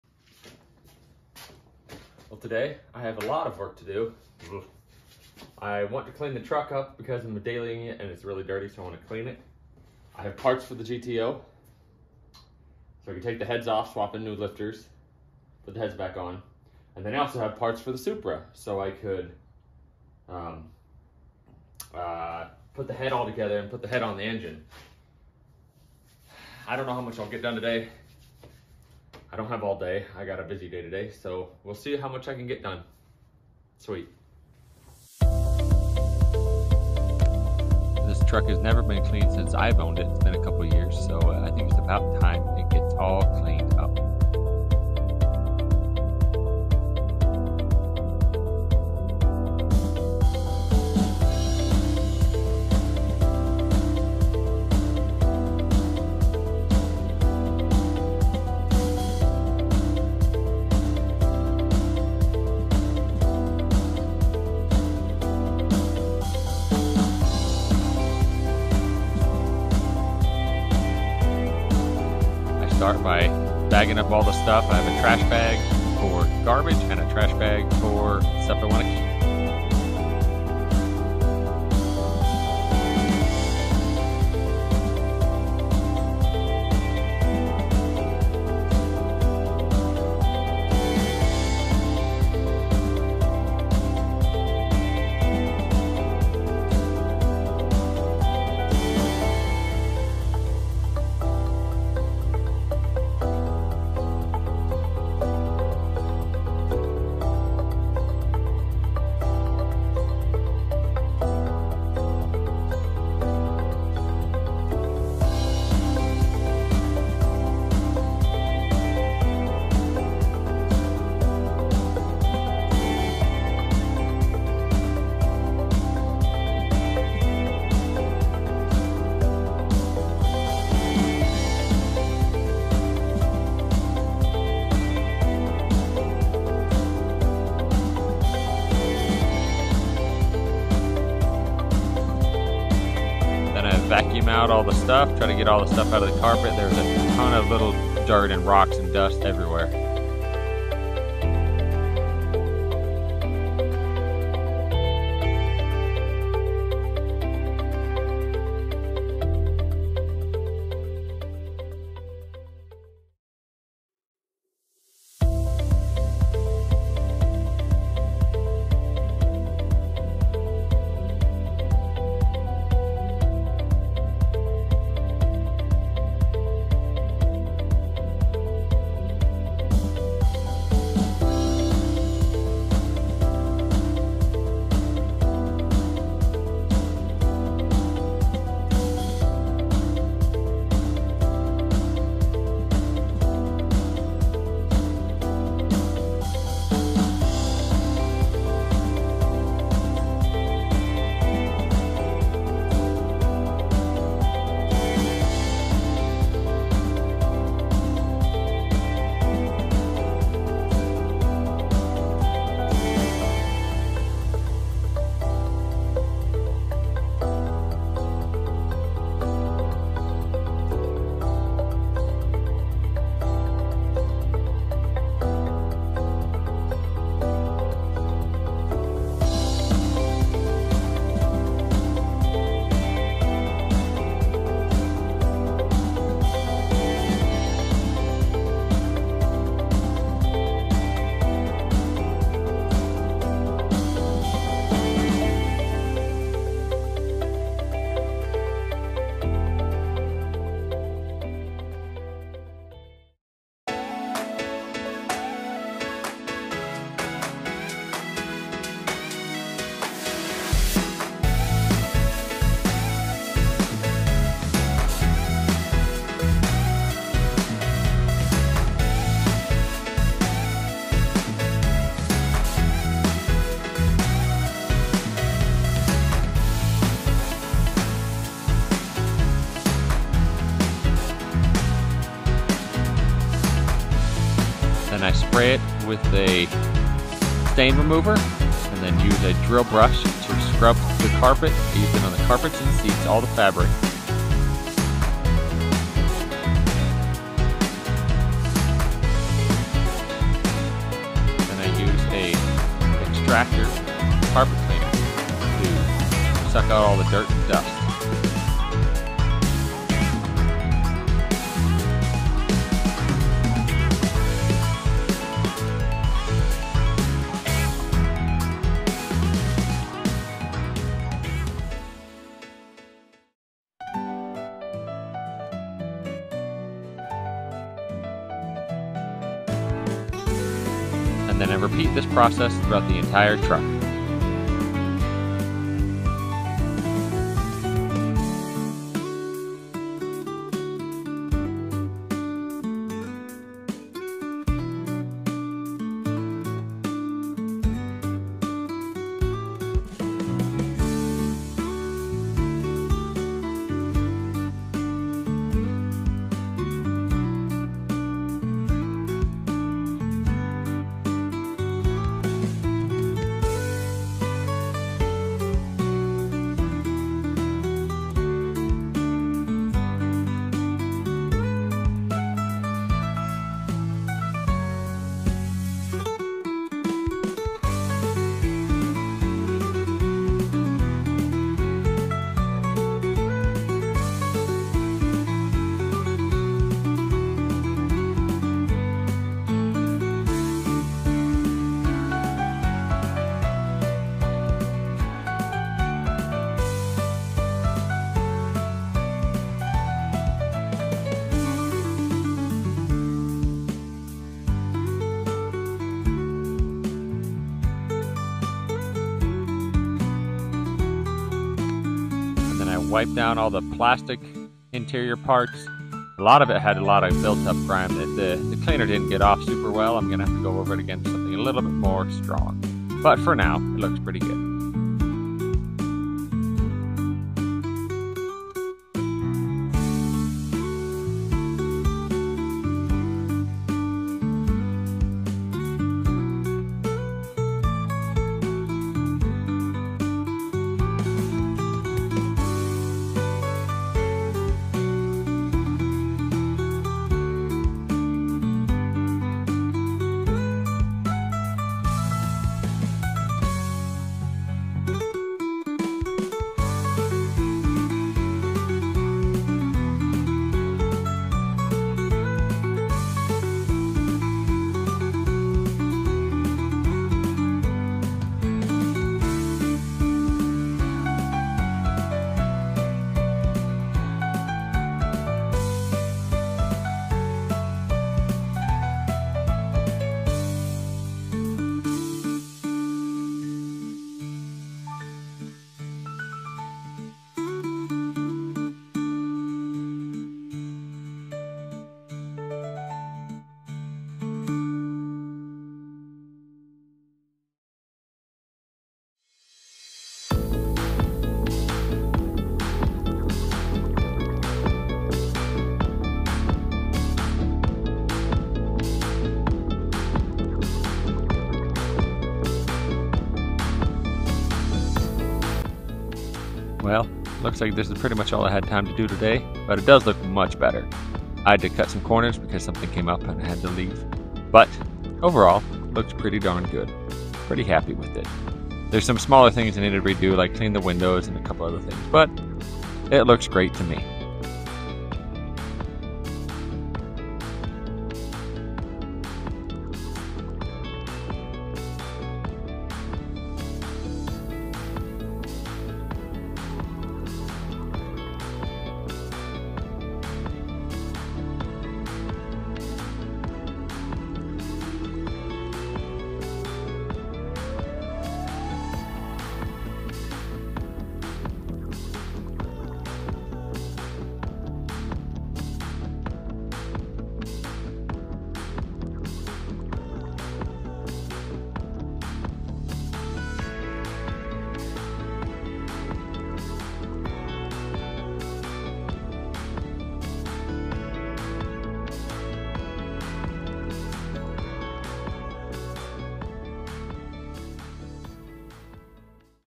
Cleaning my 24 year old truck for the first time!
Cleaned the interior for the first time since I’ve owned it. Not perfect but definitely way better!